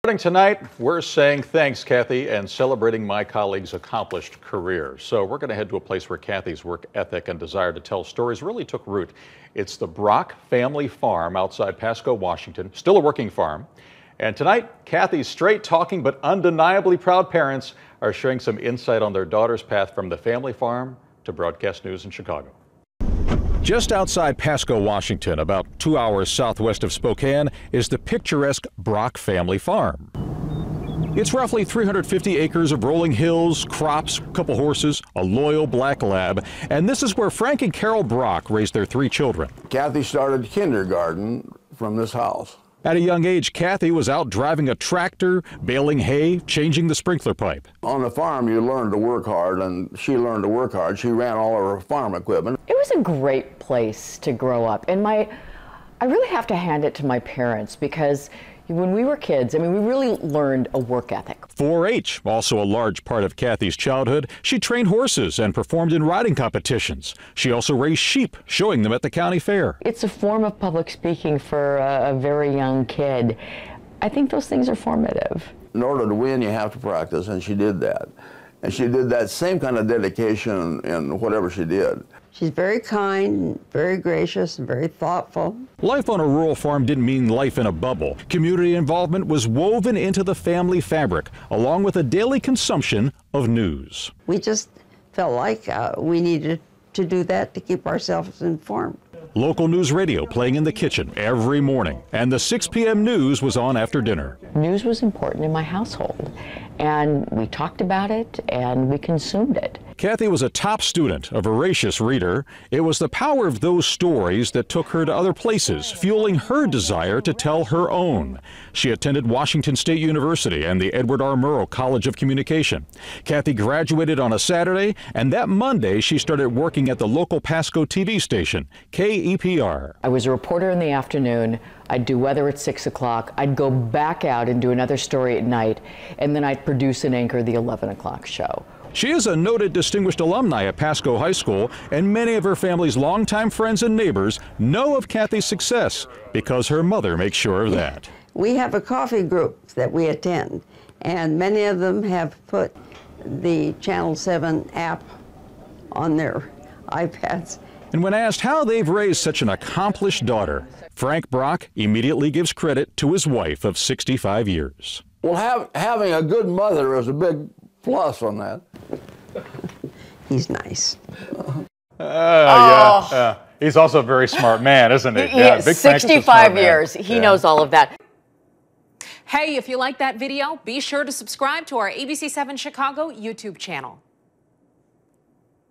At the family farm, Kathy Brock's passion for stories was born
The Brock family farm outside Pasco, Washington is still a working farm. Kathy's straight-talking but undeniably proud parents shared some insight on their daughter's path from the farm to the newsroom.